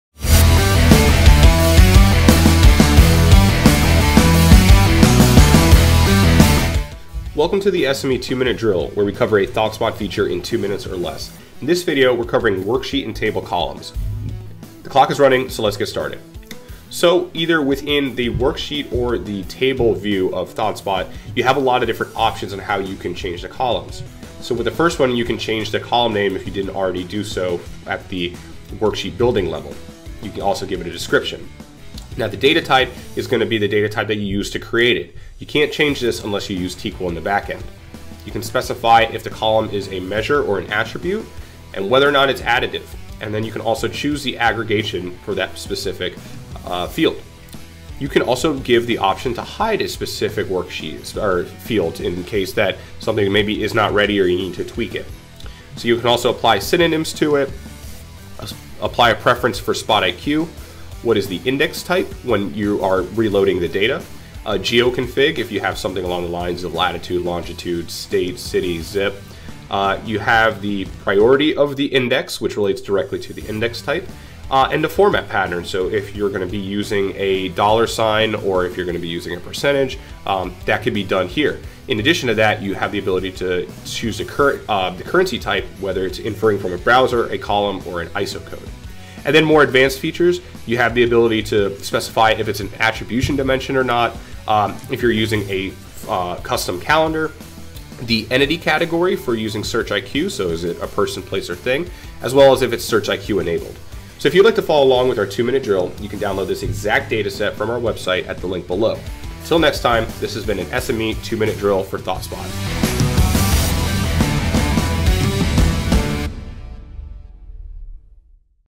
ThoughtSpot Worksheet and Table Columns
Modeling includes setting basic information for a data column such as its name, description, and type.